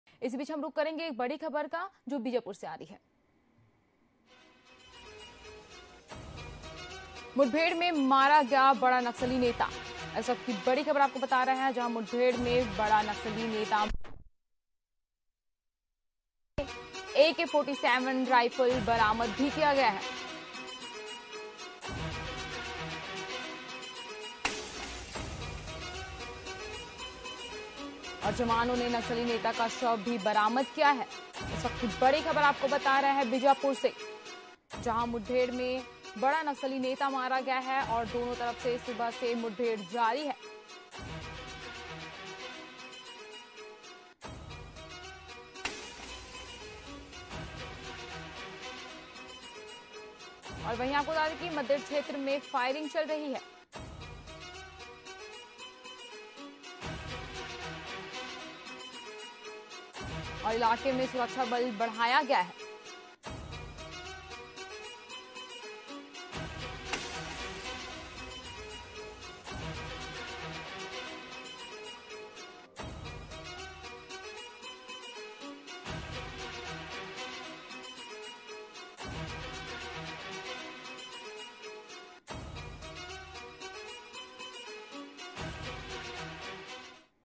Chhattisgarh Breaking Today || मुठभेड़ मारा गया बड़ा नक्सली नेता, जवानों ने किया शव बरामत
Video Tilte :-मुठभेड़ मारा गया बड़ा नक्सली नेता, जवानों ने किया शव बरामत